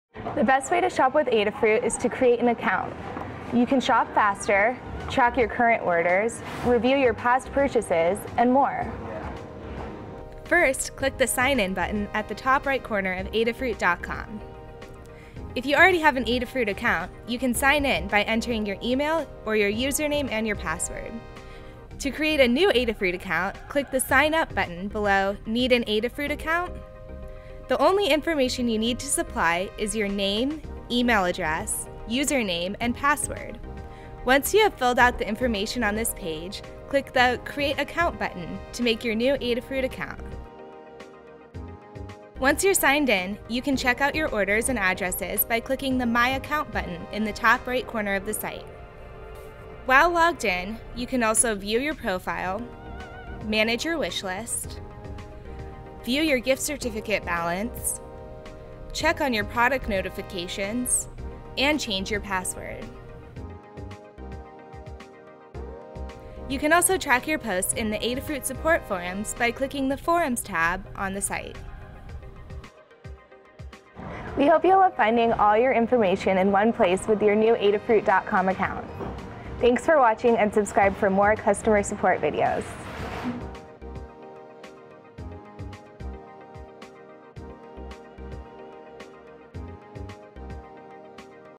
How to Create an Adafruit Account
Learn why and how to create an account on Adafruit.com!